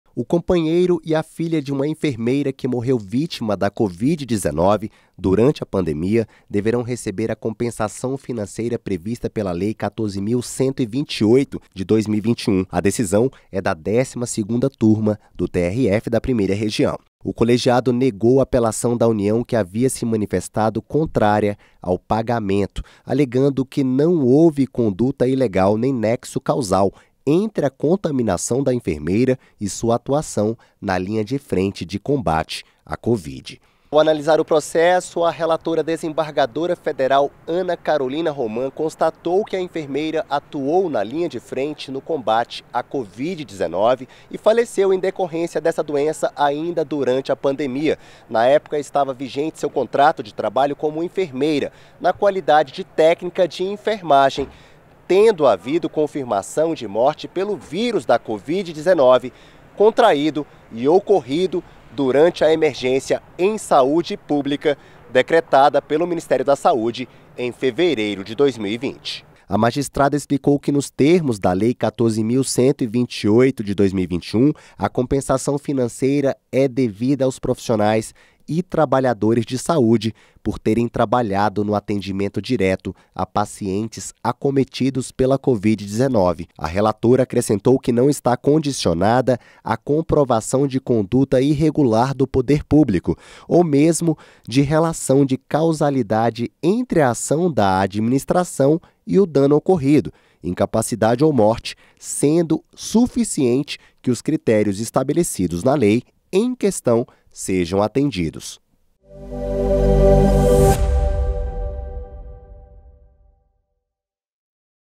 Mantida sentença que determinou compensação financeira a família de enfermeira falecida na pandemia
O companheiro e a filha de uma enfermeira que morreu vítima da Covid-19 durante a pandemia deverão receber a compensação financeira prevista pela Lei n. 14.238/2021. Assim entendeu a 12ª Turma do Tribunal Regional Federal da 1ª Região (TRF1) ao negar a apelação da União que havia se manifestado contrária ao pagamento da compensação.

Acompanhada à unanimidade pela Turma, a relatora, desembargadora federal Ana Carolina Roman, constatou que a enfermeira atuou na linha de frente no combate à Covid-19 e faleceu em decorrência dessa doença ainda durante a pandemia. Na época, estava vigente seu contrato de trabalho como enfermeira, na qualidade de técnica de enfermagem, tendo havido confirmação de morte pelo vírus da Covid-19 contraído e ocorrido durante a Emergência em Saúde Pública decretada pelo Ministério da Saúde já em fevereiro de 2020.

Nos termos da Lei nº 14.128/2021, a compensação financeira é devida “aos profissionais e trabalhadores de saúde que durante o período de emergência de saúde pública de importância nacional decorrente da disseminação do novo coronavírus (SARS-CoV-2) por terem trabalhado no atendimento direto a pacientes acometidos pela Covid-19 ou realizado visitas domiciliares em determinado período de tempo, no caso de agentes comunitários de saúde ou de combate a endemias, tornaram-se permanentemente incapacitados para o trabalho ou ao seu cônjuge ou companheiro, aos seus dependentes e aos seus herdeiros necessários em caso de óbito”, explicou a magistrada.

A União havia alegado que houve inadequação da via eleita e falta de interesse processual no processo em razão da ausência de pedido na esfera administrativa. A apelante defendeu que não houve conduta ilegal nem nexo causal entre a contaminação da enfermeira e a sua atuação na linha de frente de combate à Covid, além de existirem “imprecisões na lei em relação à responsabilidade fiscal, como ausência de estimativa de impacto orçamentário ou indicação de fontes de recursos”.

Todos esses argumentos foram combatidos pela magistrada que, em primeiro lugar, rejeitou a alegação de inadequação da via eleita, “uma vez que a falta de solicitação administrativa prévia não impede o ajuizamento da presente demanda em atenção ao princípio da inafastabilidade do Poder Judiciário (art. 5º, XXXV, da Constituição Federal)”.

A desembargadora também não aceitou a preliminar de falta de interesse processual, considerando legítima a busca da concretização dos direitos dos demandantes em sede judicial.

Ressaltou a magistrada, ainda, que a responsabilidade da União pela compensação financeira prevista na Lei 14.128/2021 “não está condicionada à comprovação de conduta irregular do Poder Público ou mesmo de relação de causalidade entre a ação da Administração e o dano ocorrido (incapacidade ou morte), sendo suficiente que os critérios estabelecidos na lei em questão sejam atendidos”.

“Por fim, não assiste razão à União quanto ao argumento de que a ausência de regulamentação da Lei nº 14.128/2021 seria óbice ao reconhecimento dos direitos nela previstos. O texto legal nela inserido é de tal modo suficiente que dispensa complemento por meio de decreto ou norma infralegal”, concluiu a relatora.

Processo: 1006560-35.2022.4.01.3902

Data de julgamento: 21/06/2024